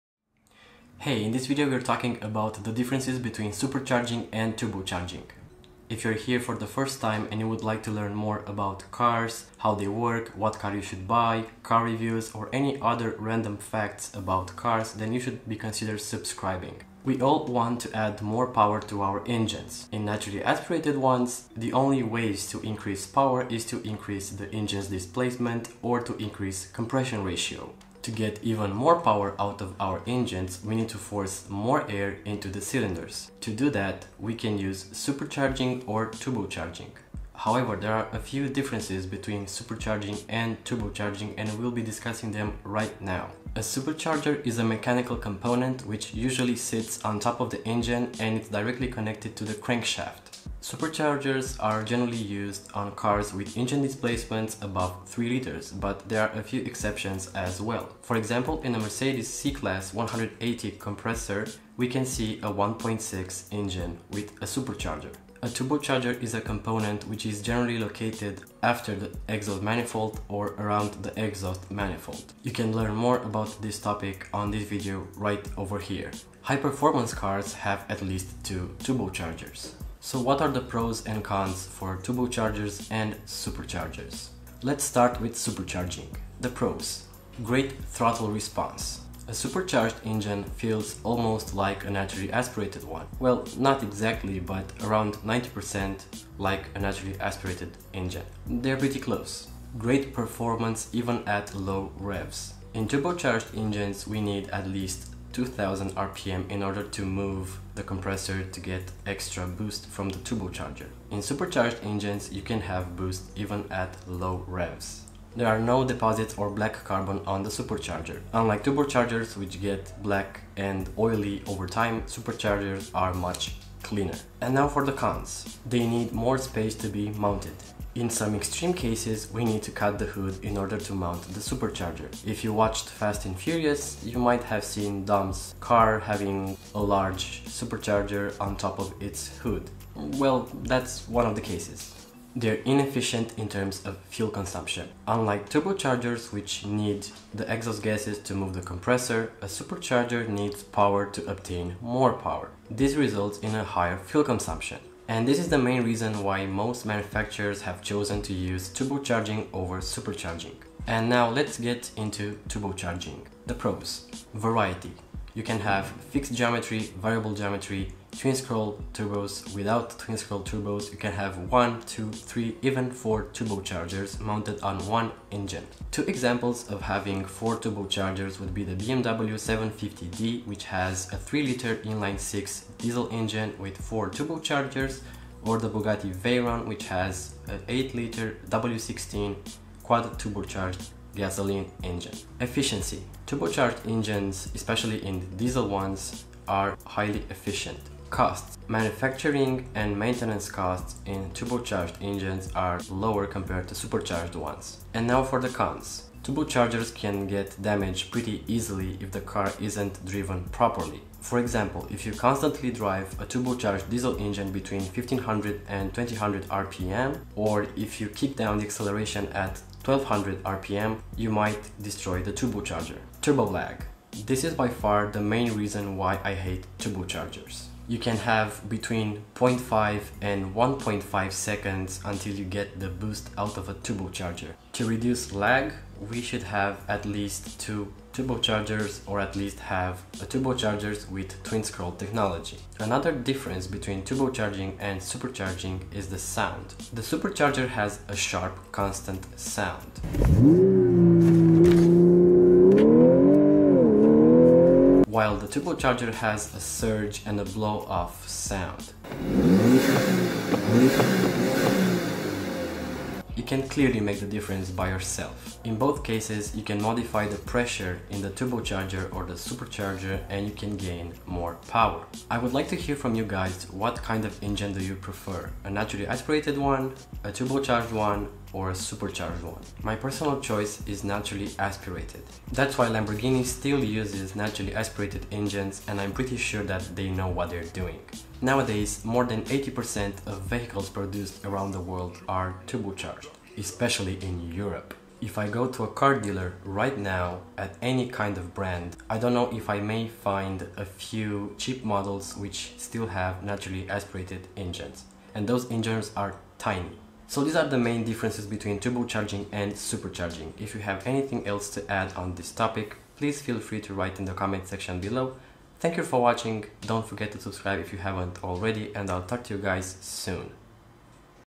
Turbocharging vs. Supercharging - What's The Difference?
Rode VideoMicro
Panasonic DMC G7

Supercharging vs. Turbocharging - What's The Difference?

We all want to add more power and torque to our engines. In naturally aspirated engines, to increase power we need to increase the compression ratio or to increase the engine's displacement.

To get even more power out of an engine we have need to force more air into the cylinders, and to do that, we can use a supercharge, a turbocharger, or both of these systems, even if it isn't the brightest idea. You can learn more about this topic in this video right over here.

A supercharger is a mechanical component, which usually sits on top of the engine, and it's directly connected to the engine's crankshaft. Superchargers are generally used on cars with engine displacements above 3.0L, but there are cases which they ca be used even on 1.6 engines such as the Mercedes C180 Kompressor.

A turbocharger is a component which is located after the exhaust manifold or around the exhaust manifold. An engine can have one or more turbochargers. High performance cars have at least 2 turbochargers.

So what are the pros and cons for supercharging and turbocharging?

Let's start with supercharging. The PROs:

-Great throttle response. As this component is a mechanical one and it's directly attached to the crankshaft, it responds almost instantly. Well, it's still not as good as the response found in N.A. engines, it's pretty close.

-Great performance even at low revs

-No black carbon or deposits on the supercharger. Unlike turbochargers which get black and oily over time, superchargers are cleaner.

The CONs:

-They need more space to be mounted. In some extreme cases, the hood needs to be cut in order to have in enough space for the supercharger. Remember Dom's car from Fast & Furious? Well... that's one of the cases.

-Inefficient in terms of fuel consumption - unlike turbochargers which use exhaust gases to move the compressor, superchargers need to consume power in order to get more power. This makes them inefficient, so that's why most manufacturers have chosen turbocharging over supercharging.

And now let's get into turbocharging - the PROs

-Variety - fixed geometry, variable geometry, with twin-scroll, without twin-scroll, one, two, three, even 4 turbochargers can be mounted on a car. The BMW 750d has a 3.0L Inline6 Quad-Turbocharged Diesel engine. The Bugatti Chiron and Veyron also have a 8.0L W16 quad-turbocharged gasoline engine.

-Efficiency - turbochargers are efficient, especially in Diesel engines. We just talked about this earlier.

-Costs - manufacturing and maintenance costs are lower in turbocharged engines compared to supercharged ones

CONS

-Turbochargers can get damaged easily if the car isn't driven accordingly. For example, if you constantly drive a Diesel engine at 1500 to 2000 rpm or if you kickdown the engine at 1300rpm, you will eventually destroy the turbocharger.

-Lag - this is by far the main reason why I don't like turbochargers. To reduce lag, an engine should have at least 2 turbochargers or one turbocharger with Twin-scroll technology.

Another difference between supercharging and turbocharging is the sound. The supercharger has a sharp constant sound. while the turbocharger has a surge and a blow off sound.

Nowadays, over 80% of engines produced around the globe are turbocharged, especially in Europe. If I go to a Dealer right now, of any brand, I may find only a few cheap models which are still naturally aspirated. Most Diesels are now and twin-turbocharged.

In both cases, you can modify the pressure in the turbocharger or supercharger to gain extra power.

I would like to hear from you guys what do you prefer - a naturally aspirated engine, a turbocharged one, or a supercharged one? My personal choice is naturally aspirated. That's why Lamborghini still continues to use them and I'm sure they know what they're doing.